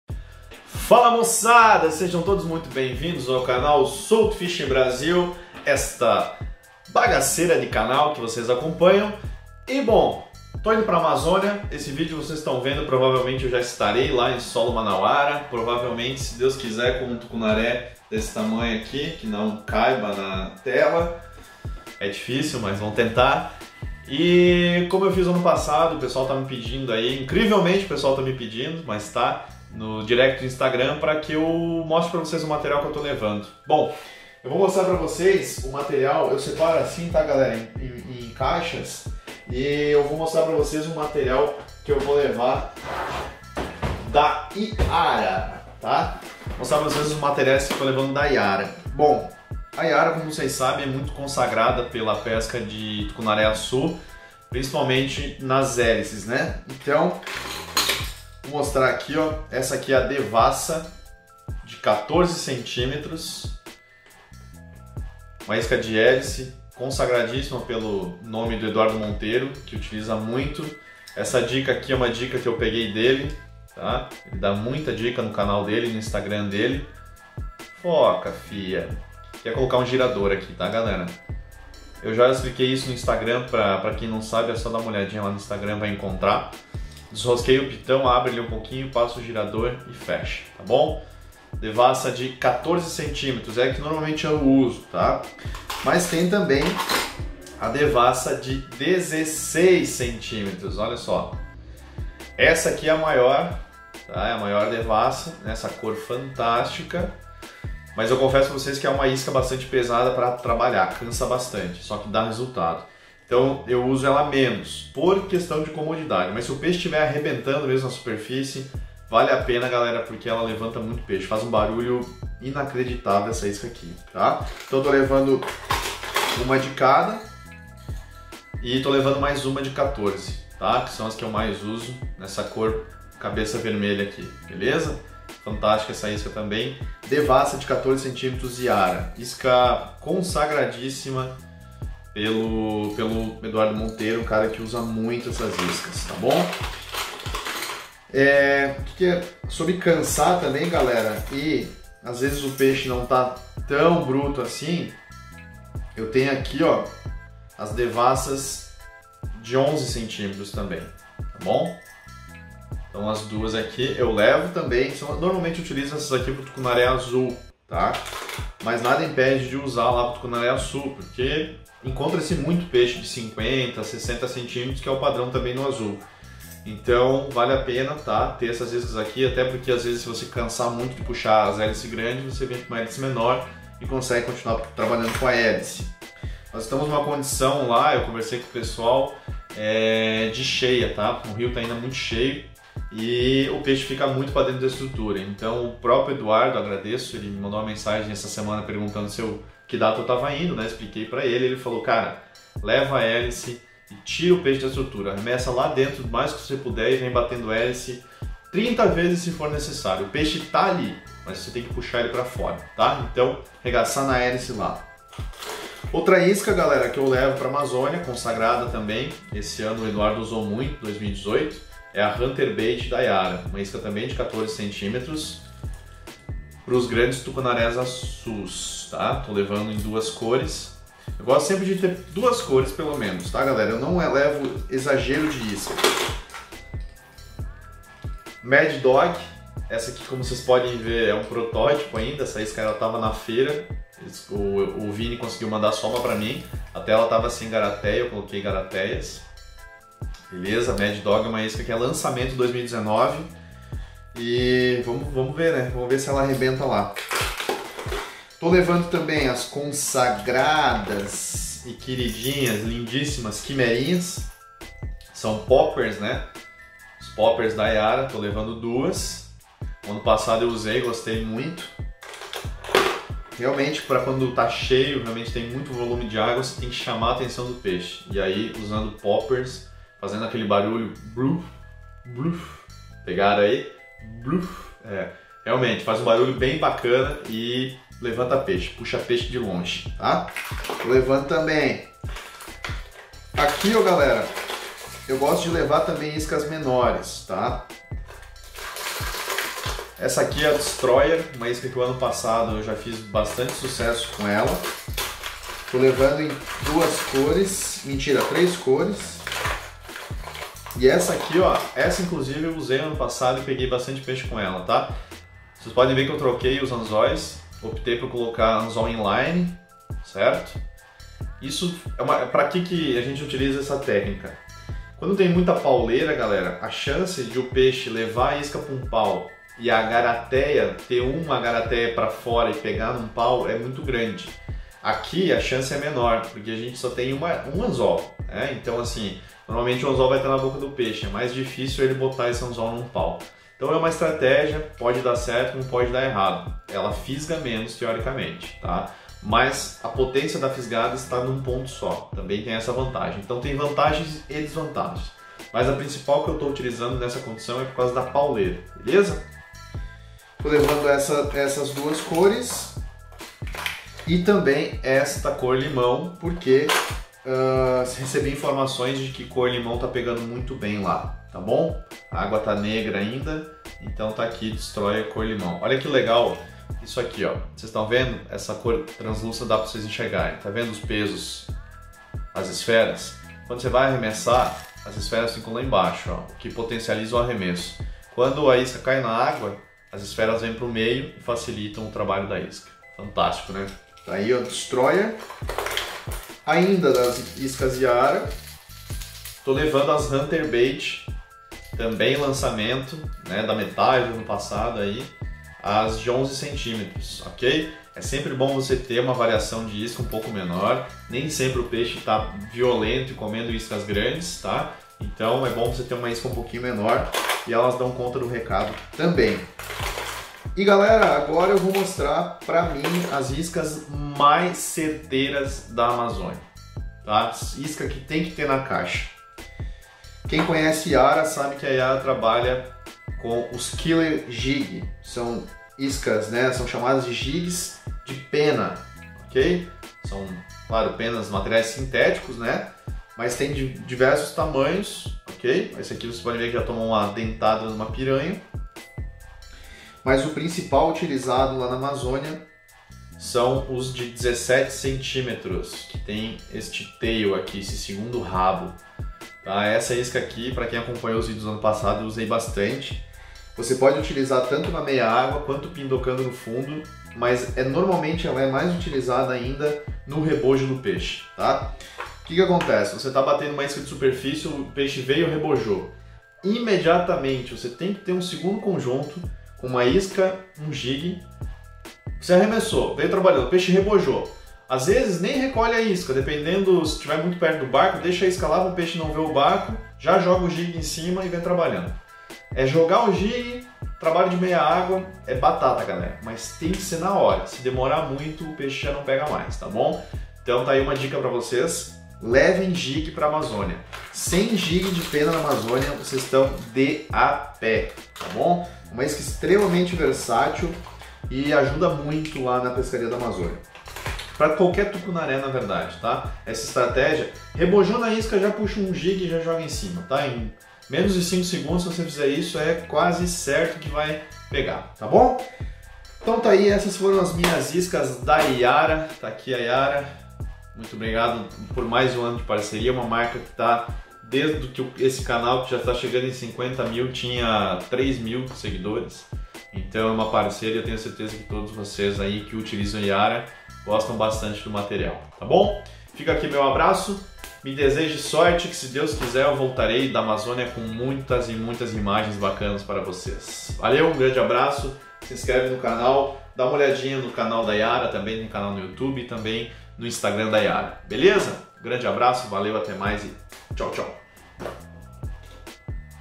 SEM JIG de Pena, você tá a pé! - Iscas para Amazônia - Yara Hélice
Faaaala moçada!
Partiu Amazônia, então segue ai as minhas iscas para essa temporada de pesca dos grandes tucunarés açus!

Já tenho data para 2020 na pousada Amazon Tucuna Expedition
04 a 10 de outubro e 18 a 24 de outubro, e digo mais, já tem 60% vendido, então corre!